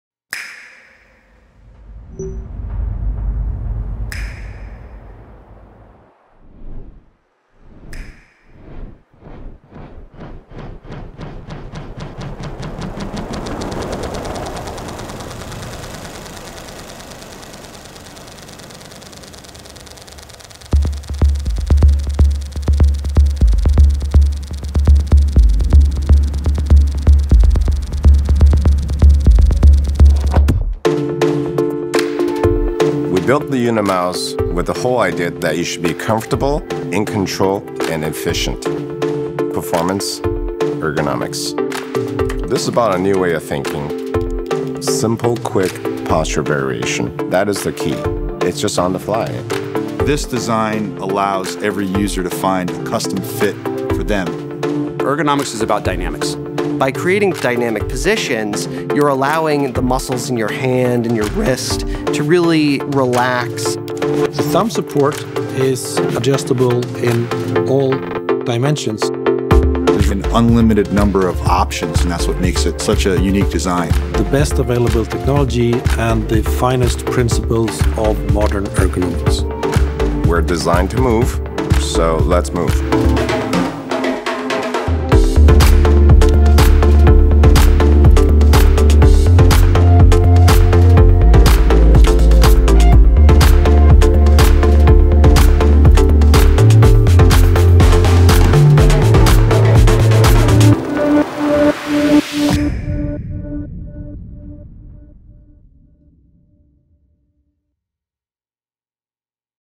Vertikalmouse: Unimouse
Entdecke die tollen Features der Contour Unimouse! Pass die Maus deiner Hand an: Du kannst die Neigung der Vertikalmouse bestimmen und die Daumenauflage auf deine Hand einstellen.